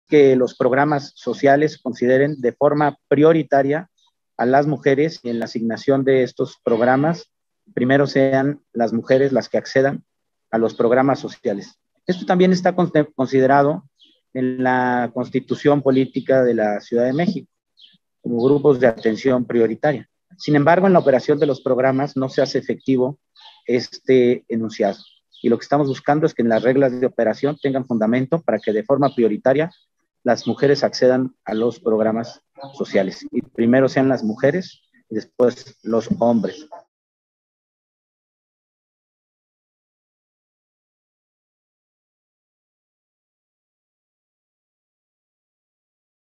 Primero las mujeres para recibir apoyos sociales
Mauricio Tabe pone en el primer lugar de la fila a las mujeres para que reciban apoyos sociales y combatir así la desigualdad que enfrenta este sector.